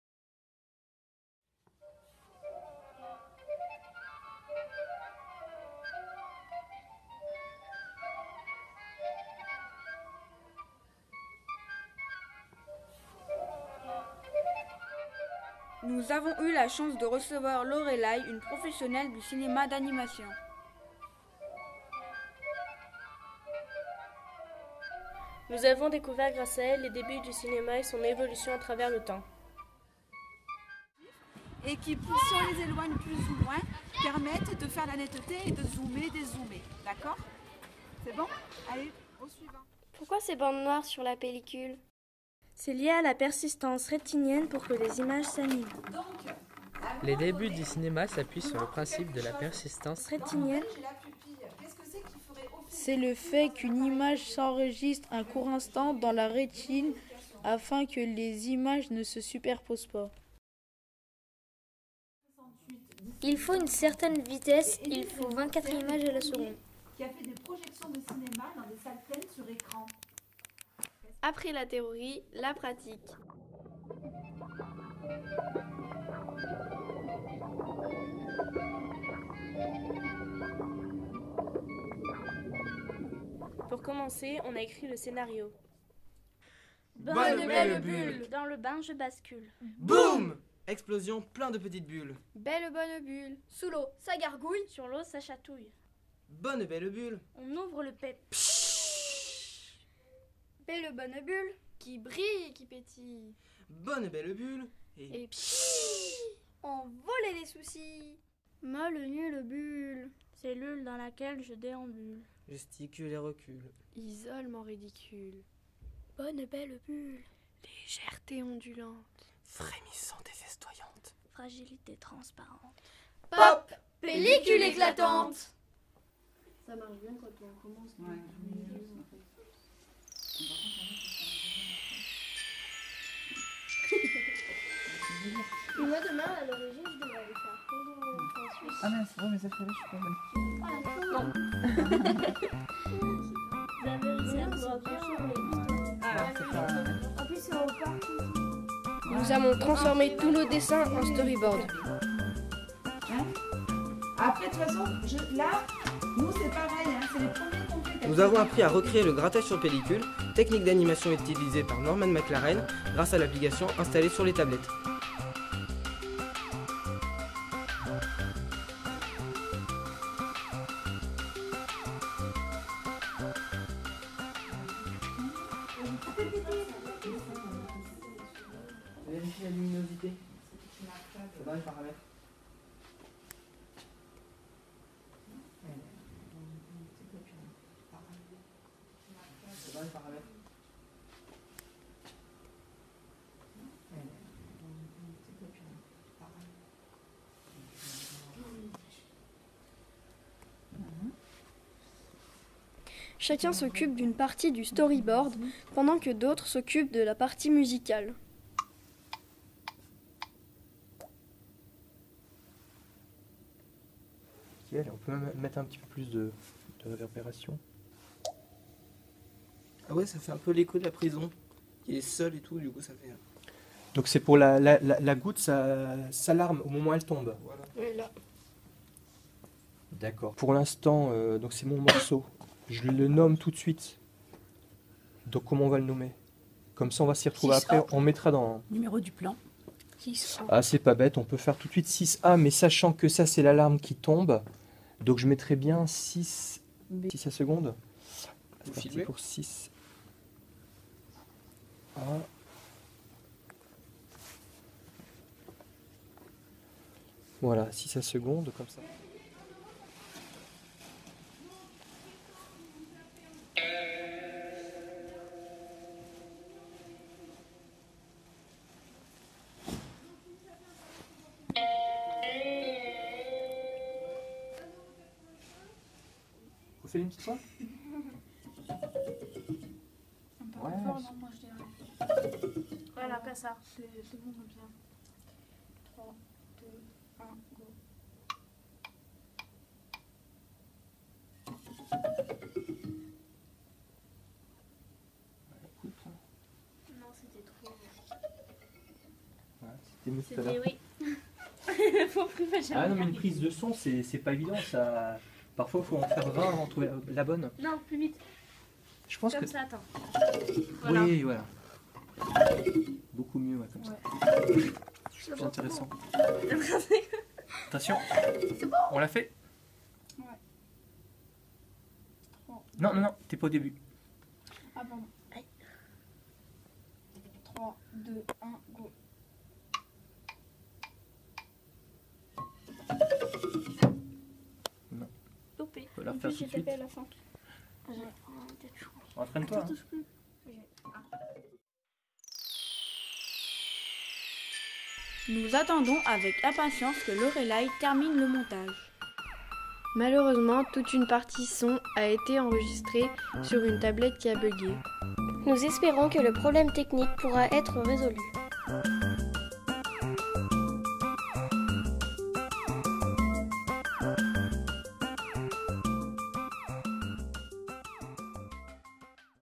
Projet "cinéma d'animation"- collège Denis Brunet (2015-2016)
Initiation aux débuts du cinéma et création d'un premier film d'animation sur le thème du mot "Bulle".